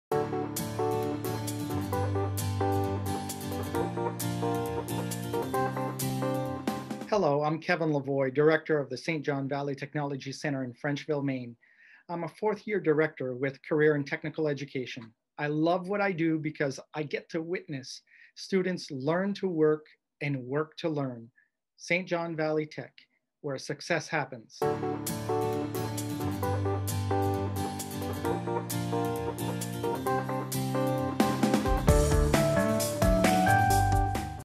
LOVESCHOOLSTAFF | Kevin Lavoie CTE Director St. John Valley Tech. Center, Frenchville
At NEWS CENTER Maine, we’re celebrating and honoring school staff and teachers by sharing their stories and highlighting their inspiring efforts amid the pandemic.